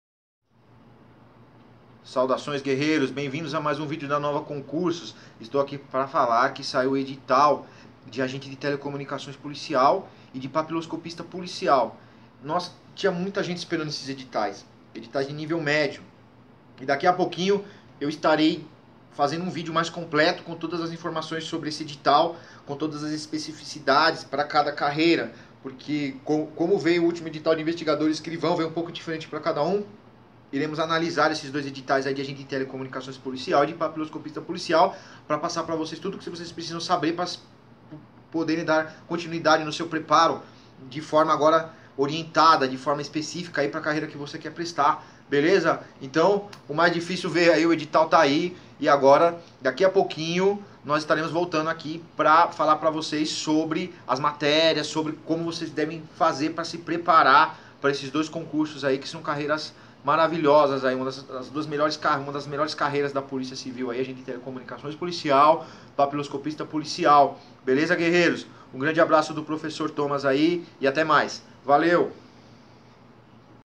PC SP 2018: Saiu Edital com 500 Vagas para Nível Médio!
🎉Saudações Guerreiros! O Edital do concurso da Polícia Civil de São Paulo com 500 vagas para Ensino Médio saiu!

👉 vagas: 500
👉 salários: até R$ 3,5 mil
👉 nível: médio
👉 banca: Vunesp
👉 inscrições: 03/05 a 01/06
👉 taxa: R$ 56,54
👉 provas: 01/07

Ainda hoje o Policial e Professor Thomas volta com novidades sobre os cargos deste concurso!